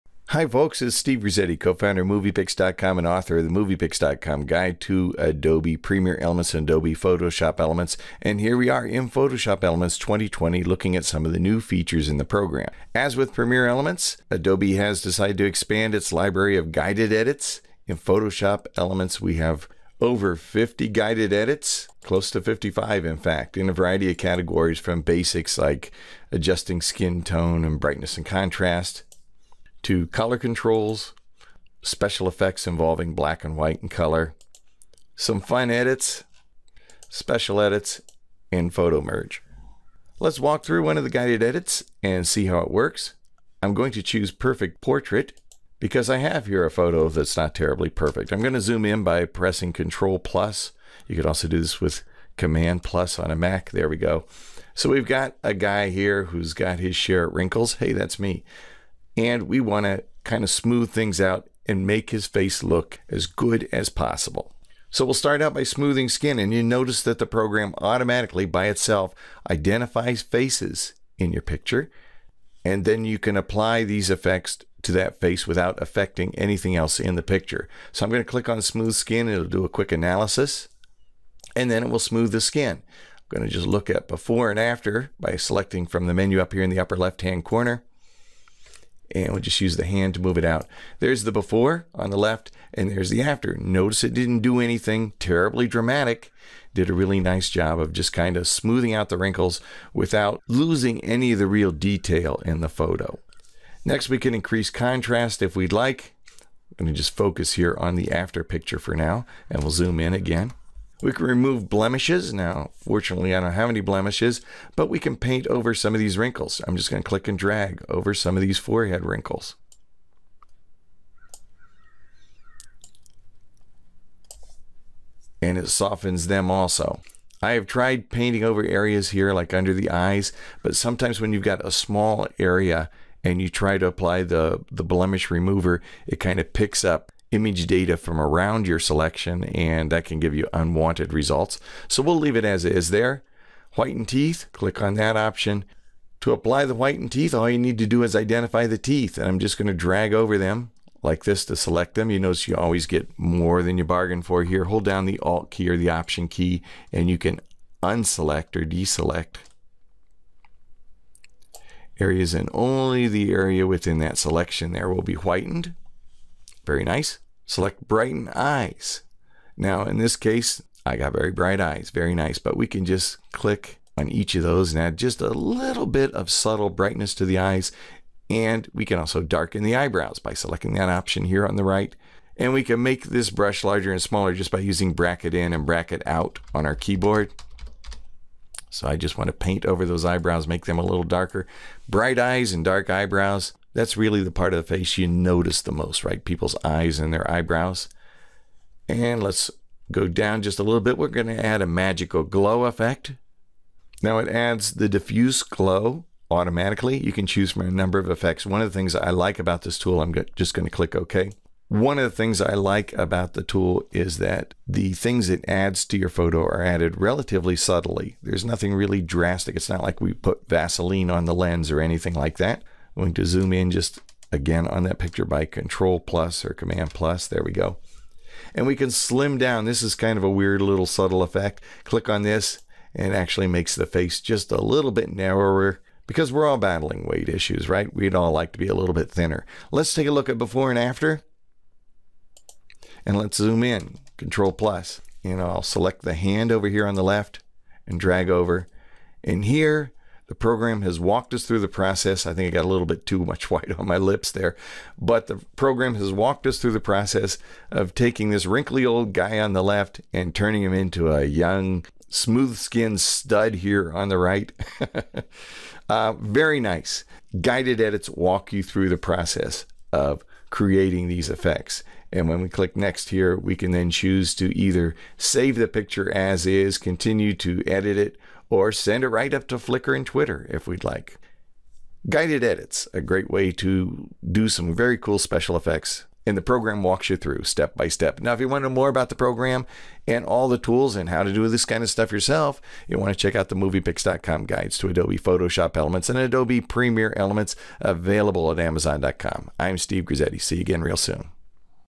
New Guided Edits in Photoshop Elements 2020
An overview of the new Guided Edits in Photoshop Elements 2020.